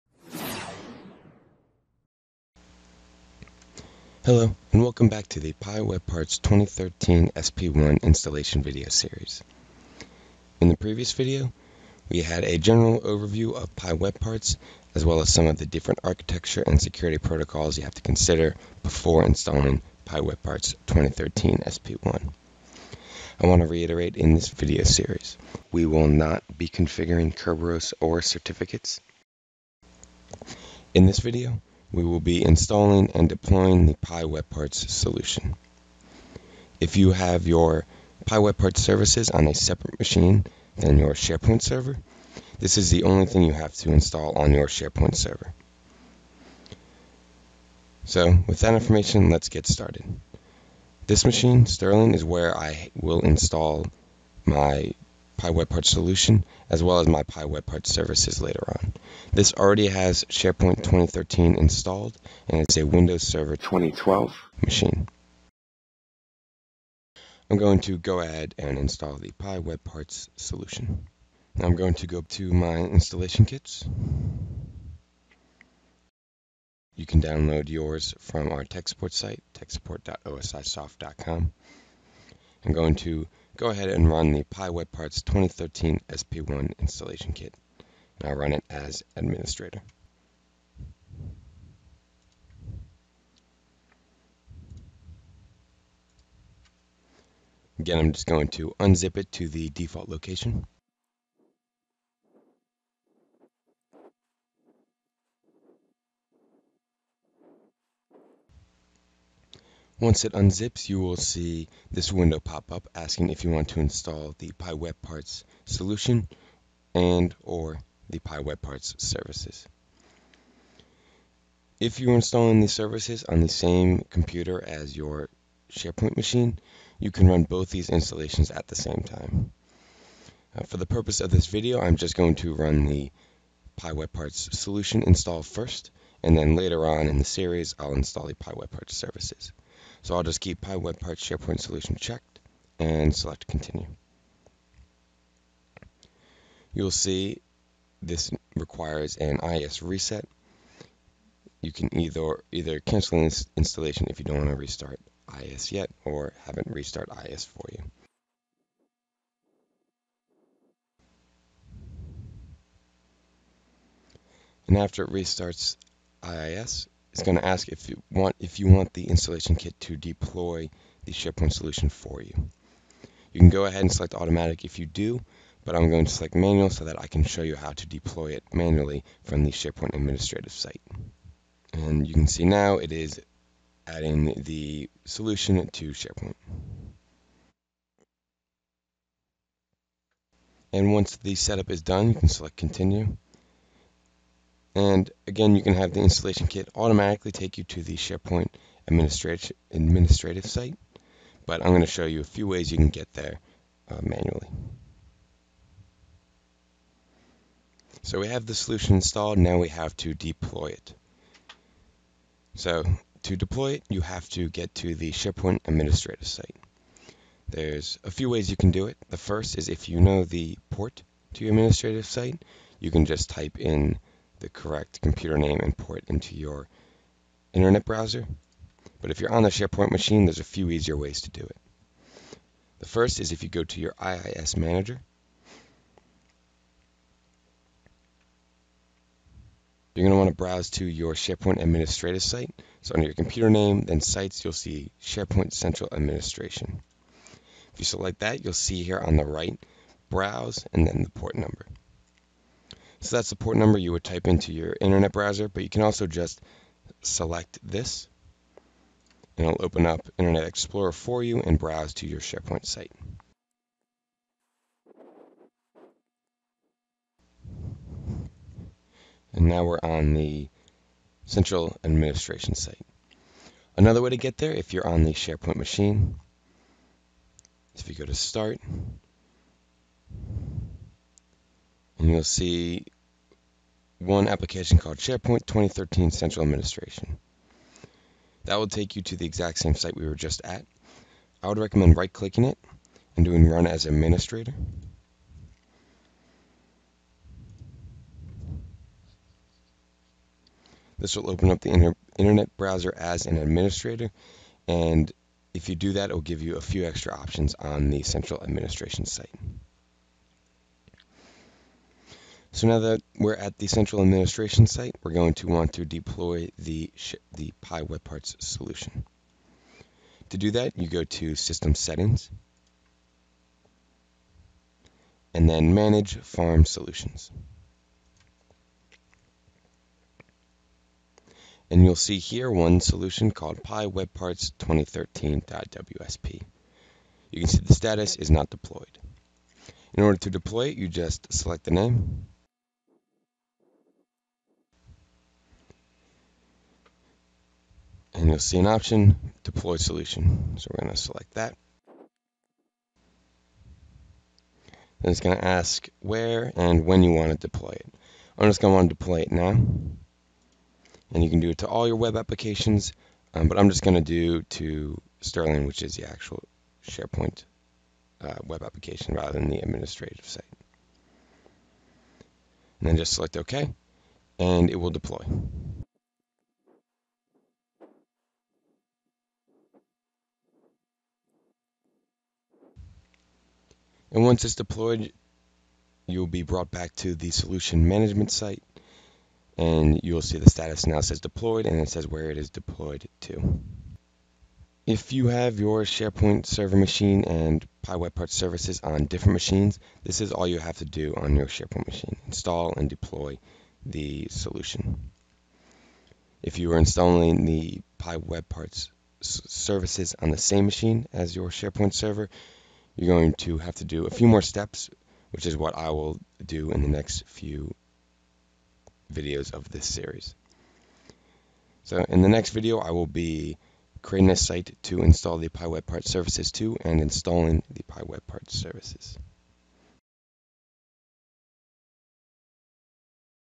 OSIsoft: PI WebParts 2013 SP1 Installation Walk-through- 2.Install and Deploy PI WebParts Solution
This video is part of a series (2/6): PI WebParts 2013 SP1 Installation Walk-through. PI WebParts is a web-based application that allows users to build and share views of data for reporting and analysis through Microsoft’s SharePoint (R) technology.
[00:50] Machine Details: Windows Server 2012 with Microsoft SharePoint 2013
[01:22] Run the install kit as the Administrator
[01:49] Use the PI WebParts Setup wizard to install PI WebParts SharePoint Solution on the SharePoint Machine
[03:33] Navigate to the SharePoint Central Administration site
[05:12] Deploy the PI WebParts Solution manually to the SharePoint site